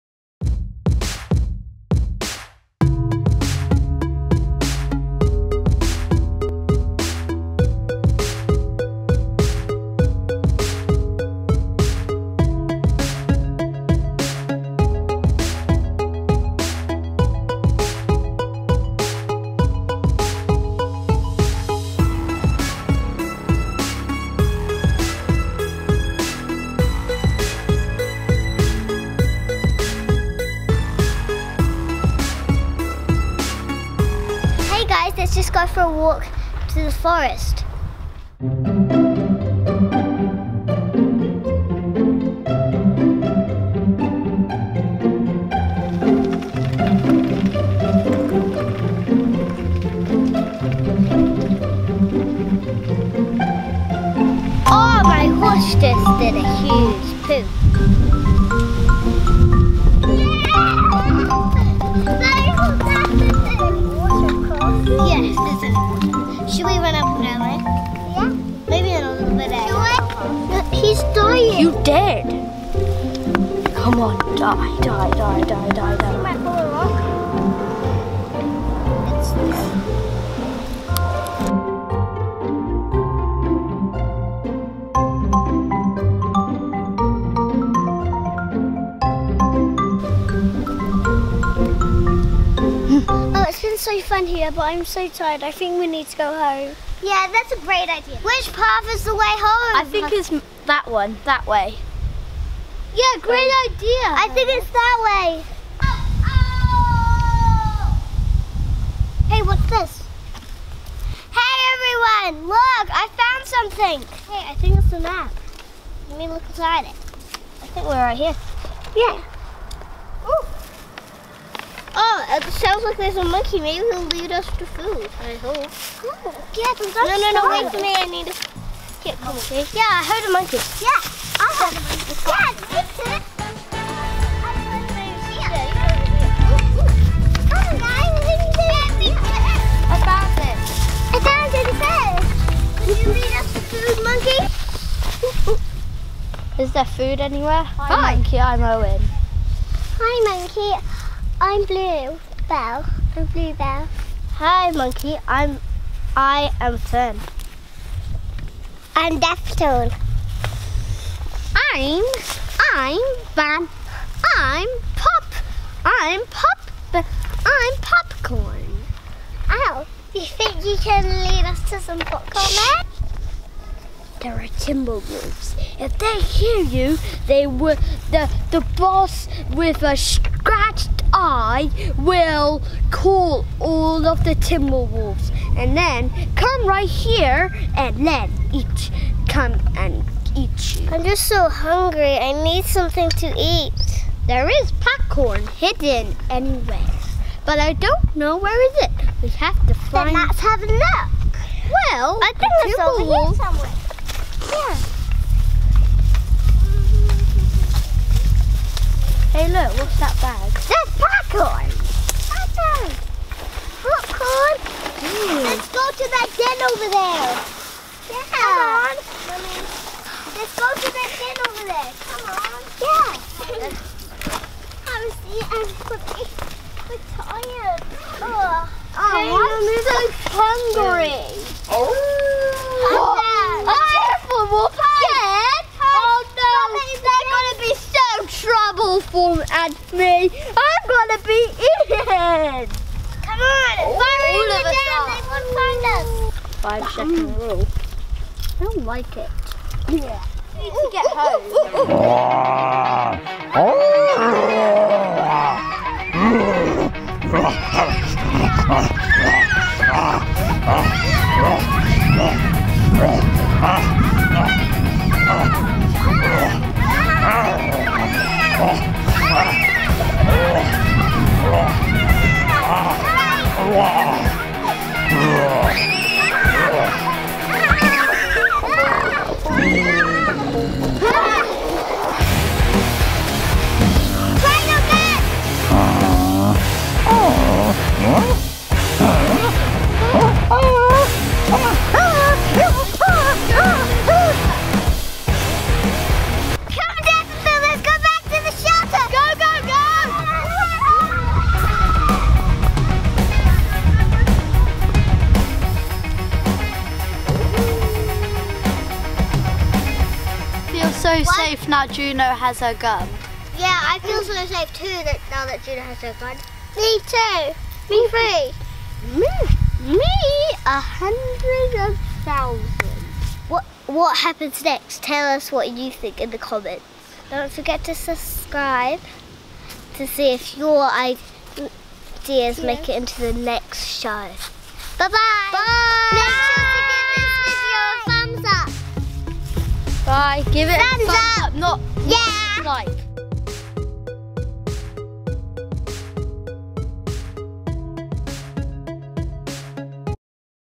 BATTLING TIMBER WOLVES IN THE FOREST
Five adventurous kids head into the forest and things go a bit wrong! WHAT HAPPENS NEXT? Leave us your ideas in the comments!